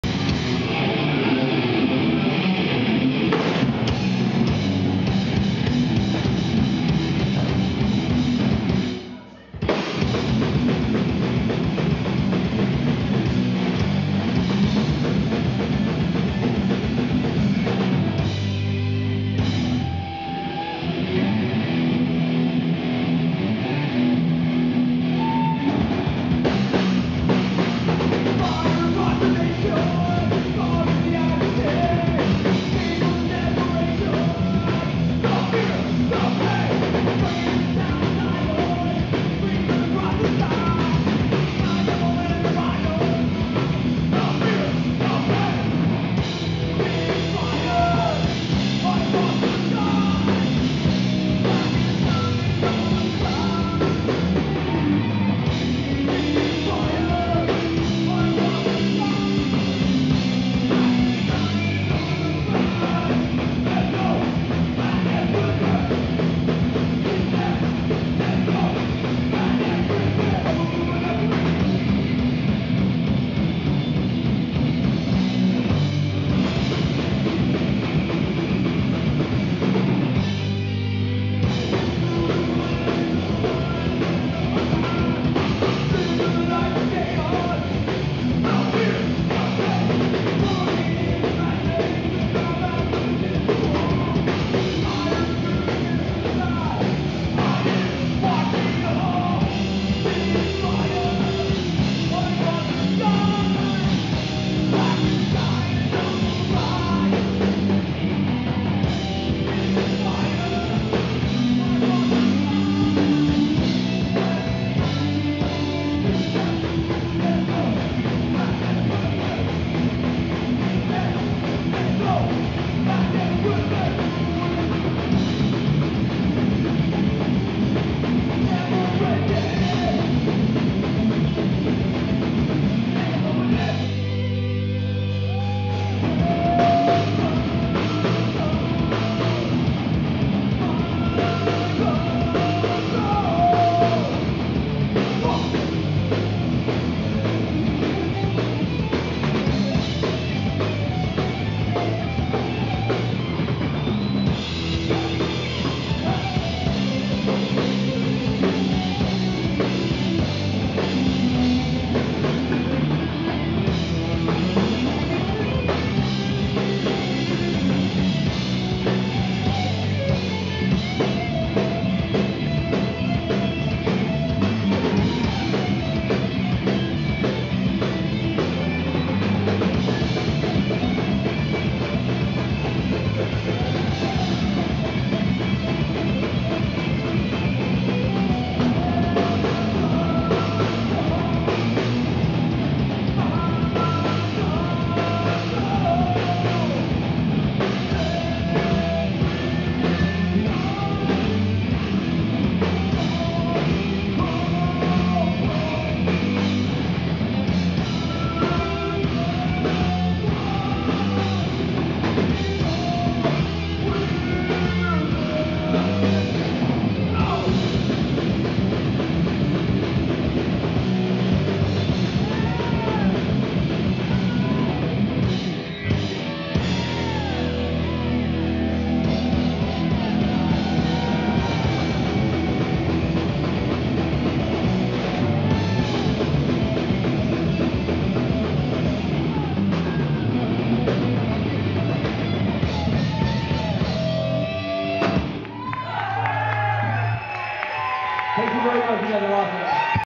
Potential Threat SF 5/12/18~4
Potential Threat SF live at the Mother's Day Metal Fest at the X Bar in Cupertino, CA. 5/12/18